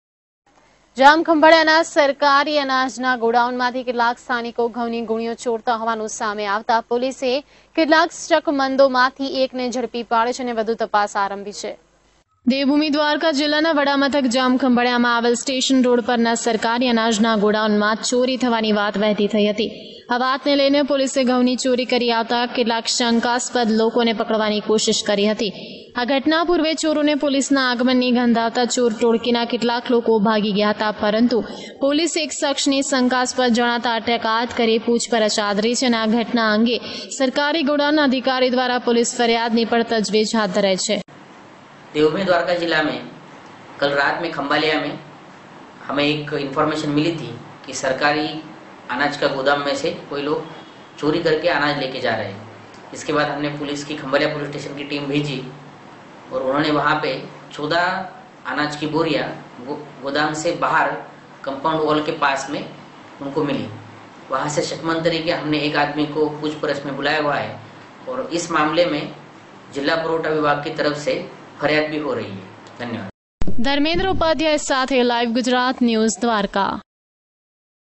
DWARKA MA ANAJ NA GODOWN MA CHORI NO PRAYAS
જામખંભાળિયાના  સરકારી અનાજના ગોડાઉનમાંથી કેટલાક સ્થાનિકો ઘઉની ગુણીઓ ચોરતા હોવાનું સામે આવતા પોલીસે કેટલાક શંકમદોમાંથી ૧ને ઝડપી પાડ્યો છે.અને વધુ તપાસ આરંભી છે.